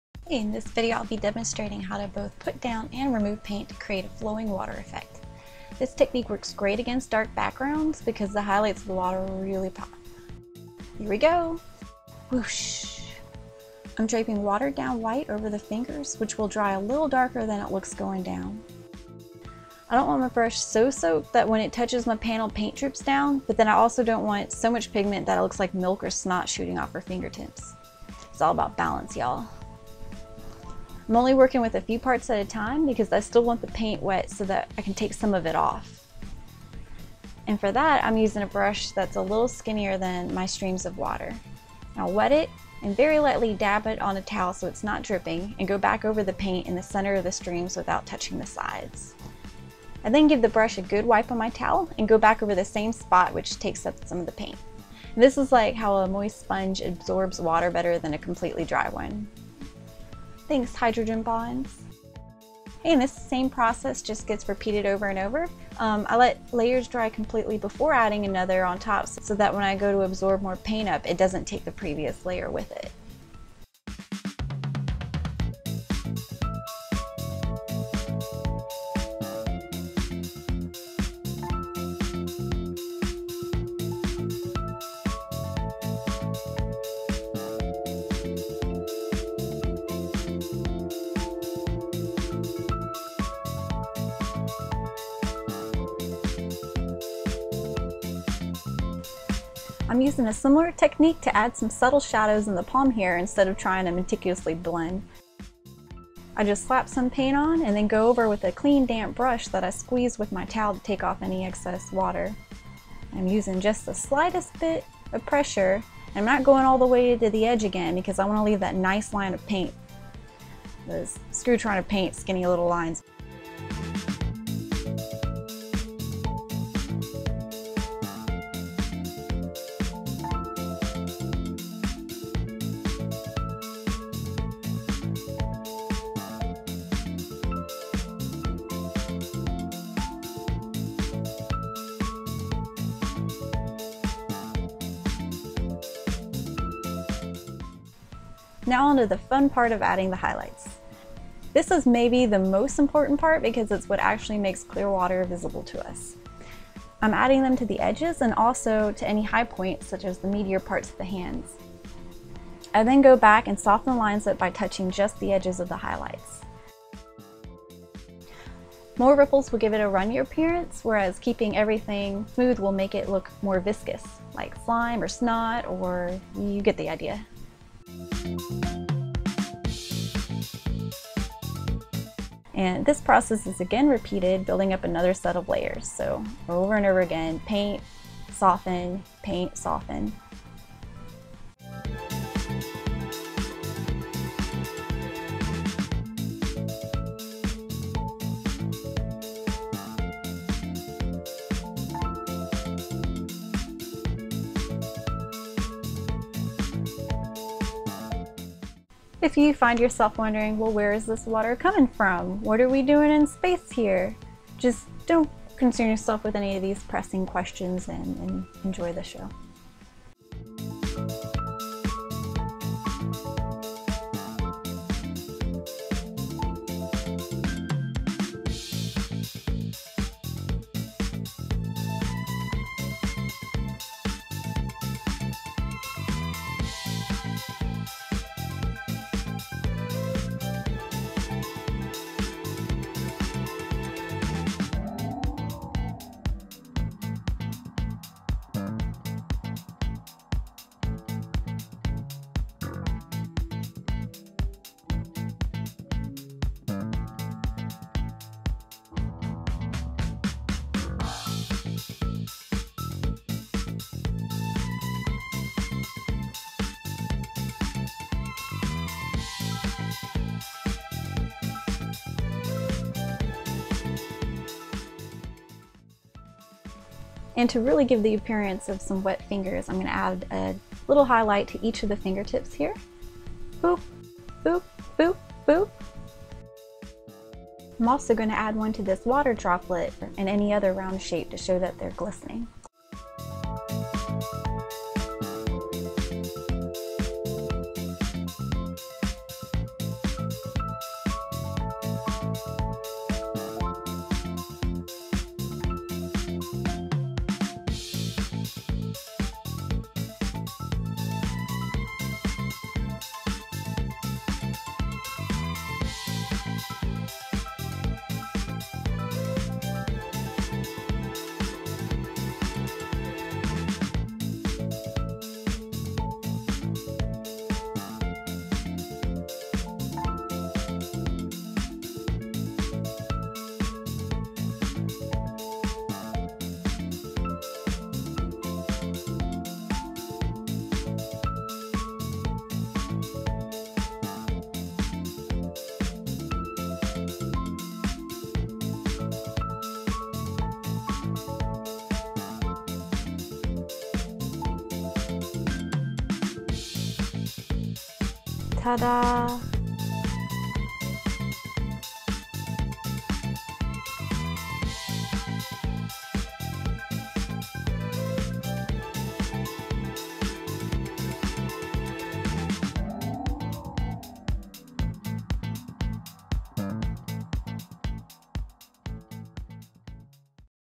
How to Paint Flowing Water Tutorial || Jacki Mayo Van Dyke | fullmetaljacki
In this video I will be demonstrating my technique of both painting and also removing paint to create a flowing water effect.